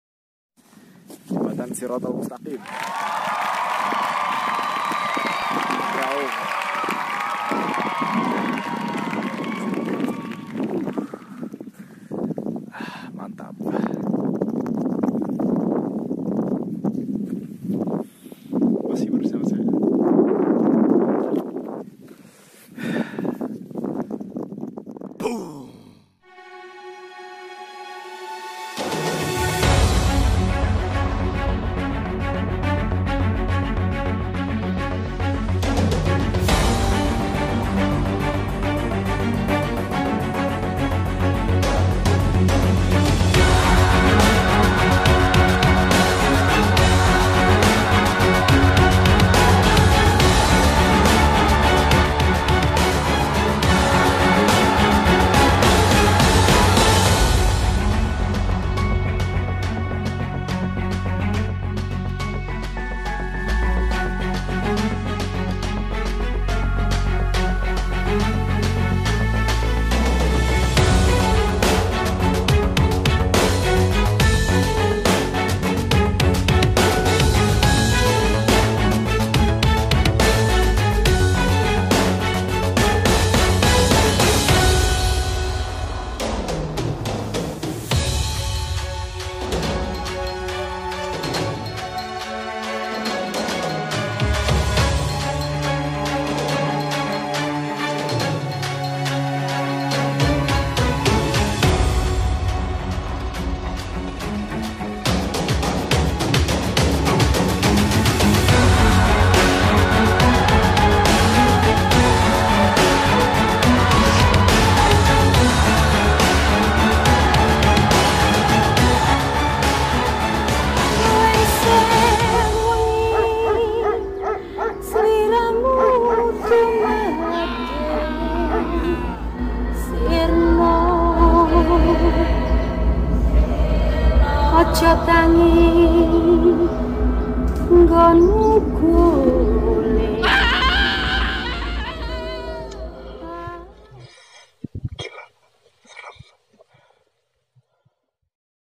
RAUNG EXTREME - The Mist Palace of the Dragon Gate Guard
Archive of DISCOVER YOUR LIMITS project - camp at 3,344 masl of Mt. Raung, Puncak Sejati

Dec 30th, 2017 - Jan 3rd 2018